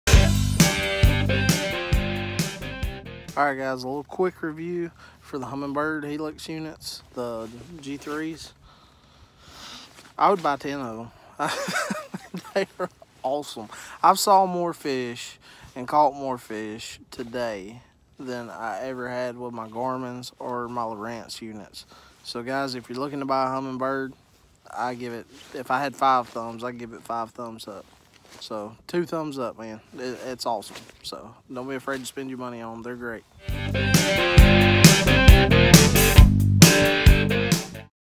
Hummingbird Helix review
In this video Pro-Angler Colton Posey gives you a quick update on whether or not he likes the new Hummingbird Helix fish finder and chart plotter!